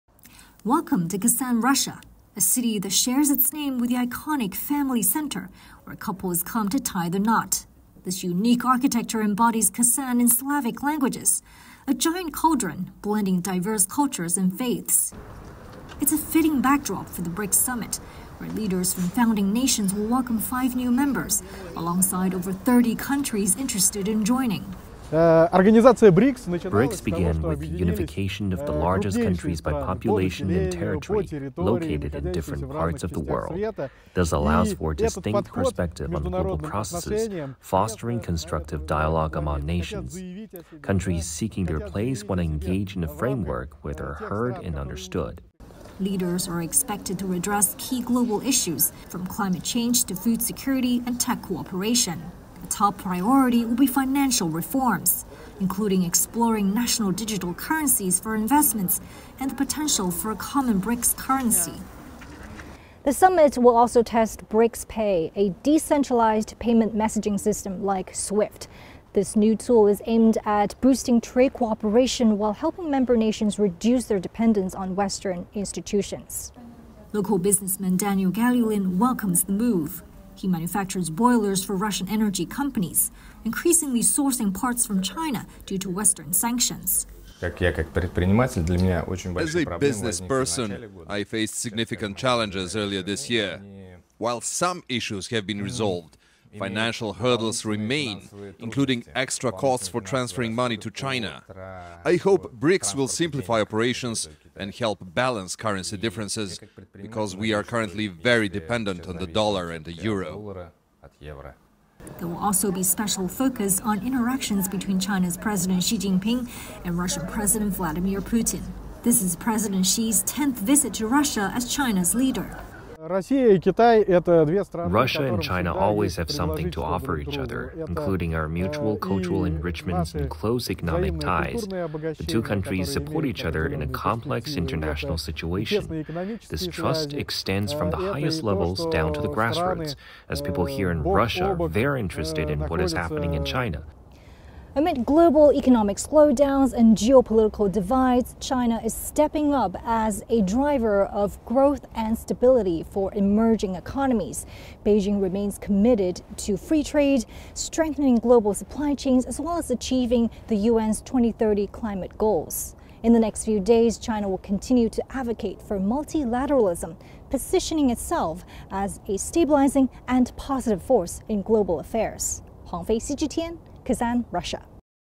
Chinese President Xi Jinping will attend the BRICS Summit in Kazan, Russia, this week
Chinese President Xi Jinping will attend the BRICS Summit in Kazan, Russia, this week, marking the group's first major gathering since its January expansion. The summit will focus on strengthening multilateralism for equitable global development and security.